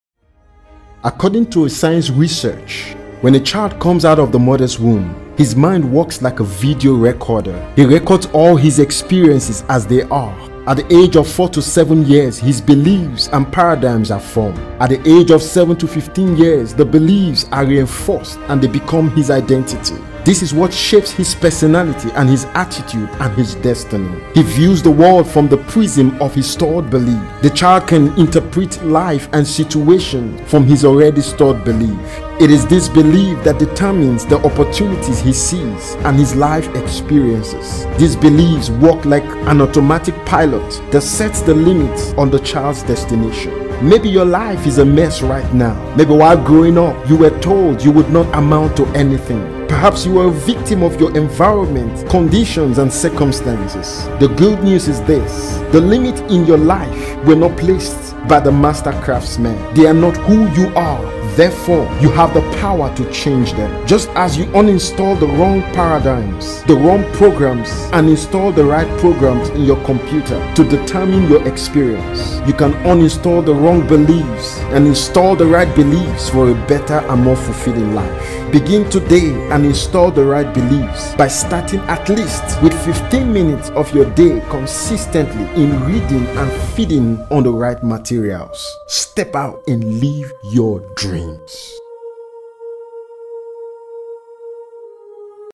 Changing limiting believes
Most of the information we received from childhood,were limiting information,this knowledge has formed believes that are now holding us back.it’s time to learn how to change them

Keep in touch with me around the web for more inspiring content